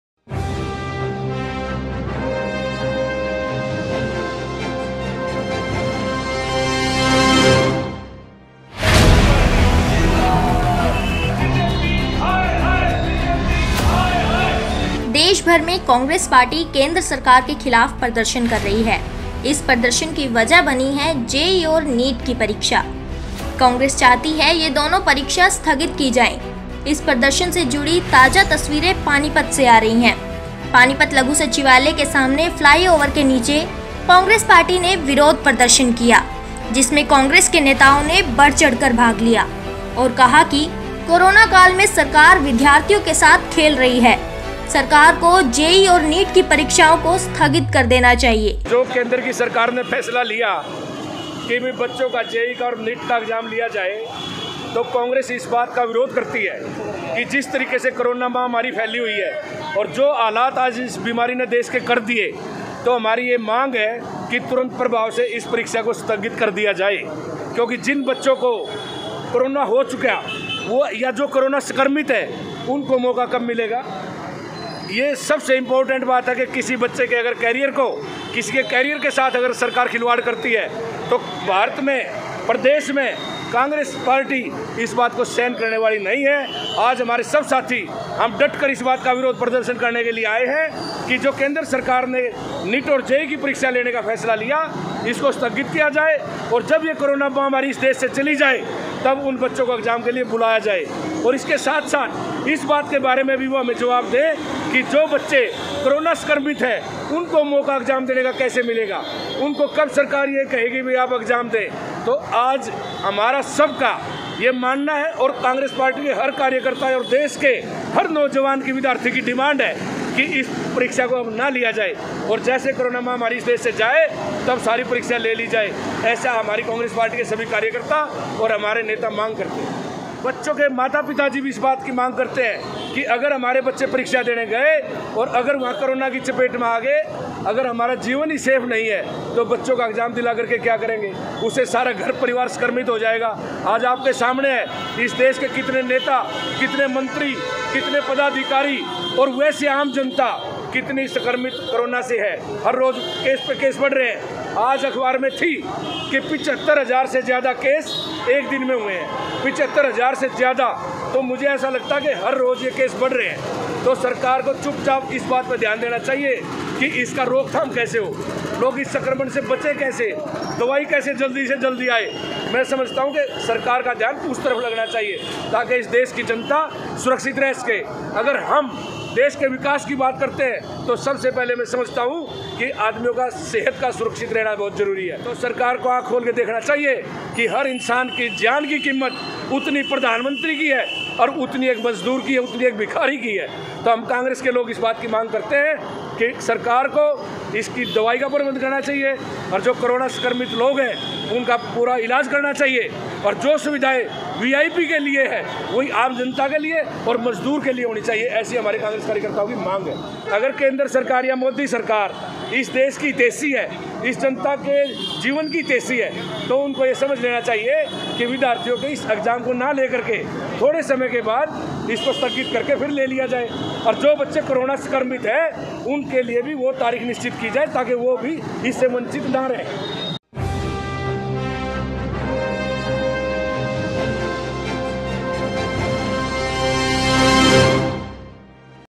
पानीपत में आज जिला कांग्रेस कमेटी ने जेई नीट की परीक्षाओं को लेकर लघु सचिवालय के सामने किया प्रदर्शन
पानीपत में आज जिला कांग्रेस कमेटी ने जेई - neet  ki परीक्षाओं को लेकर लघु सचिवालय के सामने प्रदर्शन किया जिसमें जिसमें कांग्रेस के नेताओं ने वह चढ़कर भाग लिया और कहा कि करुणा काल में सरकार विद्यार्थियों के साथ खेल रही है सरकार को जेई- neet  परीक्षाओं को स्थगित कर देना चाहिए